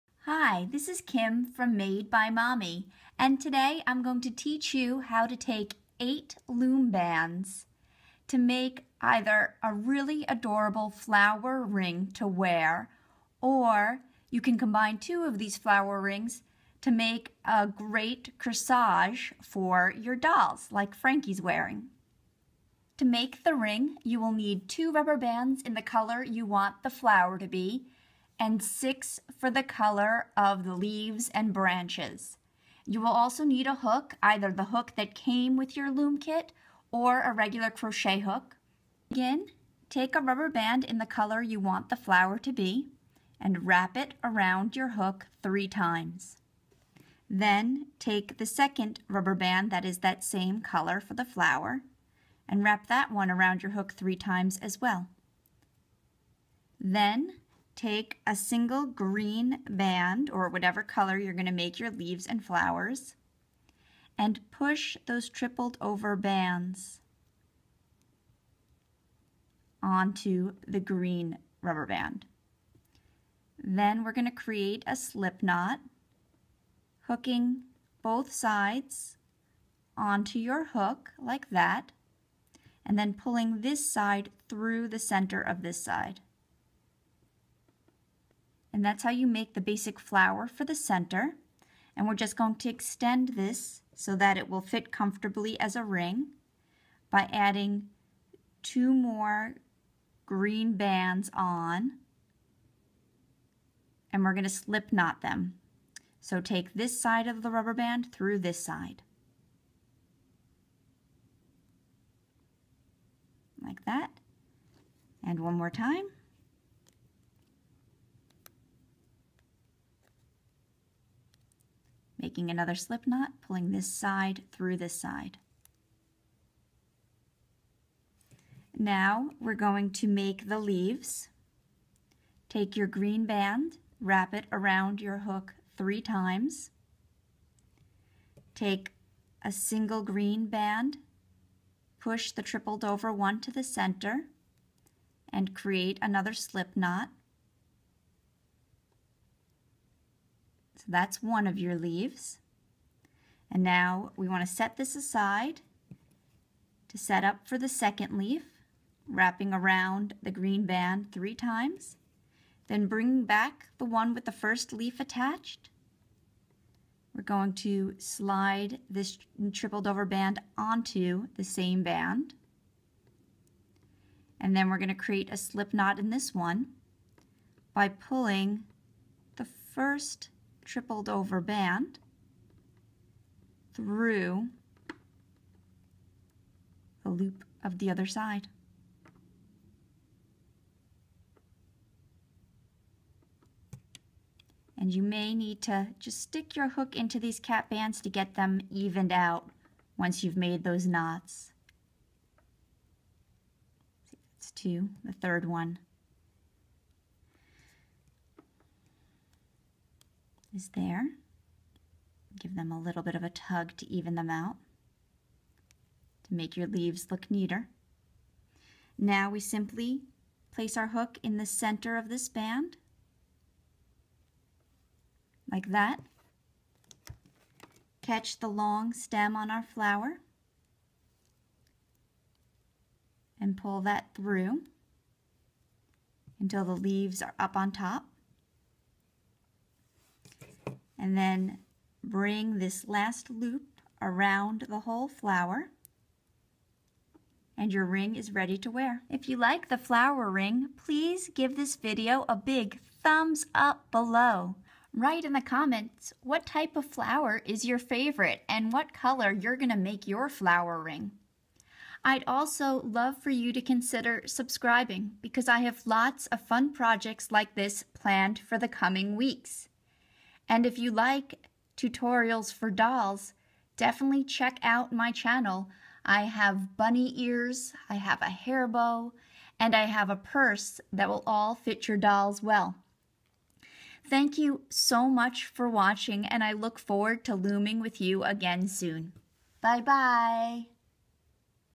Rainbow Loom Flower Ring or Doll Corsage
Tutorial showing how to use Rainbow Loom Bands to make a cute flower ring or combine a few together to make doll corsage for your Barbie, Monster High, Ever After, or American girl, or other dolls.  This design uses only 2 bands in the color of the flower and 6 for the colors of the leaves and stem.  You can use a crochet hook or the hook that came with your Rainbow Loom, CraZLoom, Wonderloom, Bandaloom, Fun Loom or other brands as well.  Copyright @2014 MadeByMommy.com

Having trouble find the Rainbow Loom band tutorial you want?  for a well organized list of all of my designs!